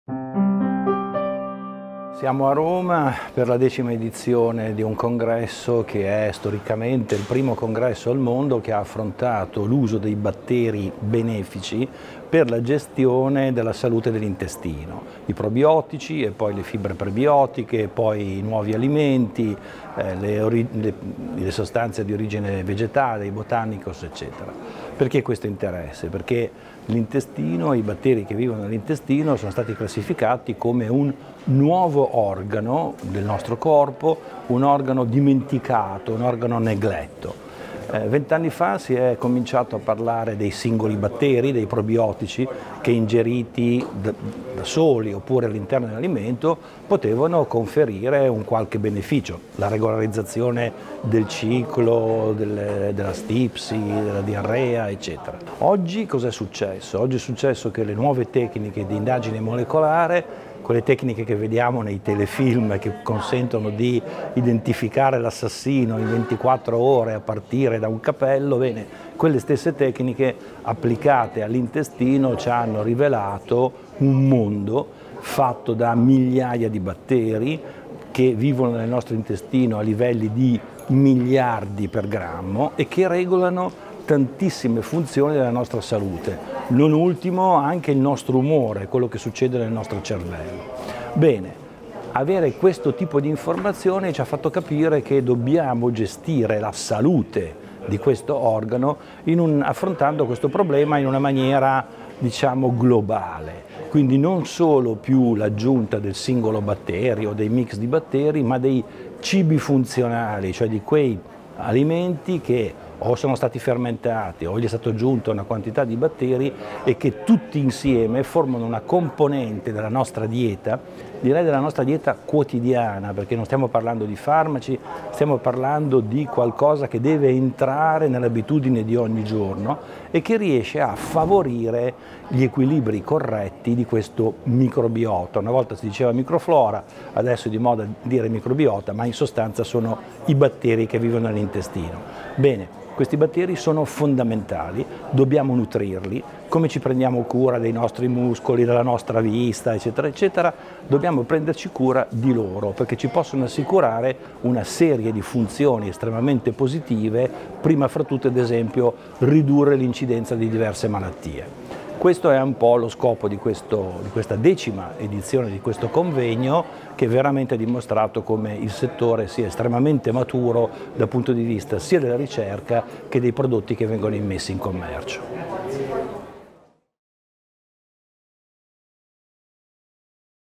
Lorenzo Morelli - Cibi fermentati e probiotici nel quotidiano
Ricevi le ultime news medico-scientifiche su microbiota e probiotici.

Dove nasce e perché tanto interesse per i probiotici, i prebiotici, i postbiotici e i nuovi alimenti? È questa la domanda che abbiamo posto al prof. Lorenzo Morelli, Direttore DiSTAS dell'Università Cattolica del Sacro Cuore Piacenza - Cremona, in occasione della 10ª edizione del congresso Probiotics, prebiotics & new foods di Roma.

Secondo Morelli, la storia inizia venti anni fa, quando si iniziò a parlare di batteri che ingeriti soli oppure all'interno di un alimento potevano conferire un beneficio per la salute come la regolarizzazione del ciclo mestruale, della stipsi o della diarrea. Tutto ciò, unito alle nuove tecniche di indagine metagenomica, oggi ha portato a riconsiderare il microbiota come un vero e proprio nuovo organo.

Per cui, è necessario introdurre nella dieta quotidiana cibi funzionali, cioè alimenti che o sono stati fermentati o vi è stata aggiunta una quantità di batteri in grado di nutrire questo organo, proprio come i muscoli o la vista, perché ci può assicurare una serie
di funzioni positive. Prima fra tutte la riduzione dell'incidenza di diverse malattie.